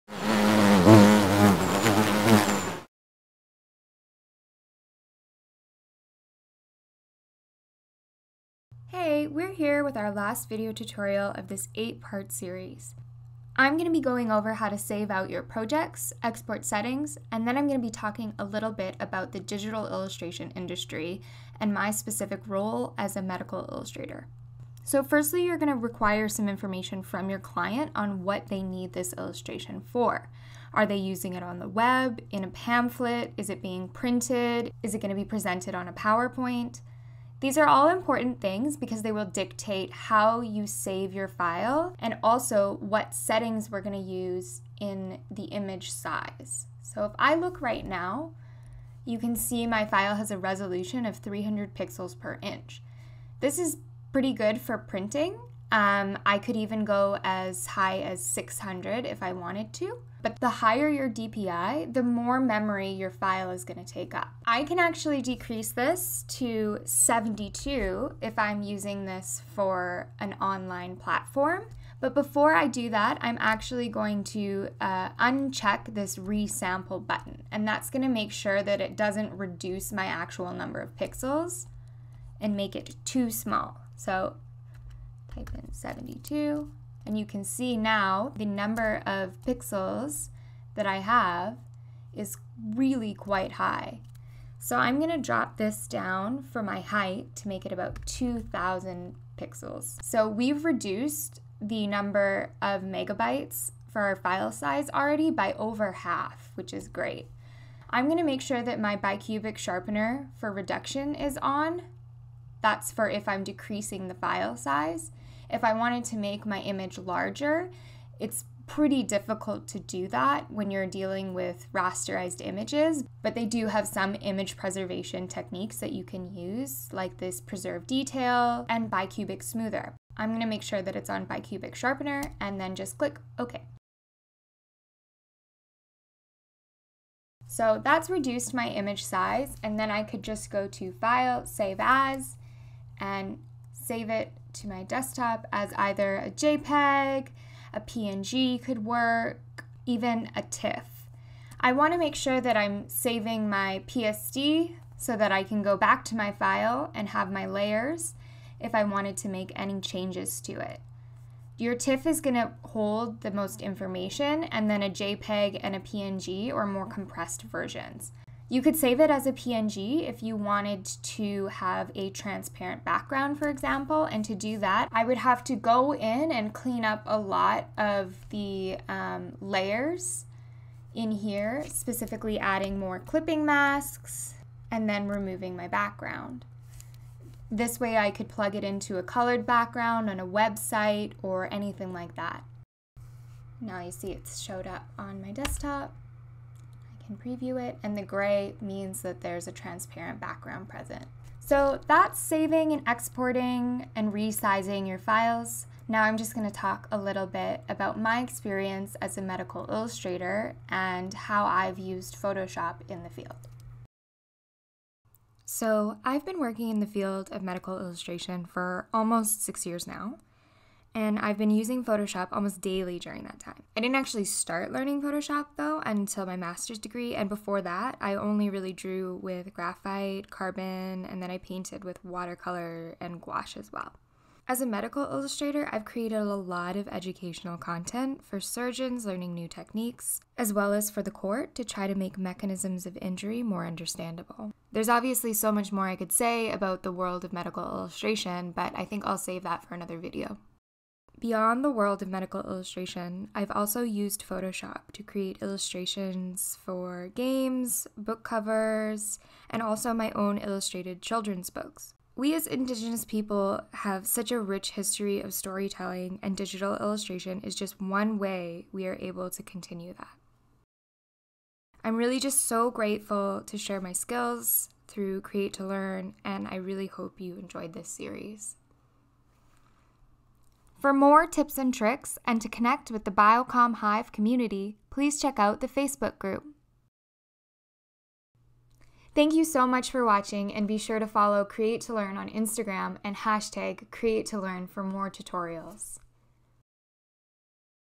Photoshop Tutorial 8 - SJ Okemow Kuzmiski
Video 8: This is the eighth and final video in a Create to Learn @ Home series with SJ Okemow Kuzmiski

If you’ve ever wanted to try out creating your own digital art on Photoshop but didn’t know where to start here’s your chance to learn the basics! SJ with Biocomm Hive brings you an 8 part video series on all you’ll need to know to get started using Photoshop.

SJ is of mixed Cree and European ancestry and currently lives in Toronto. Her work has focused on cellular animations that enable us to learn about our bodies, and thus, our connection to the world around us.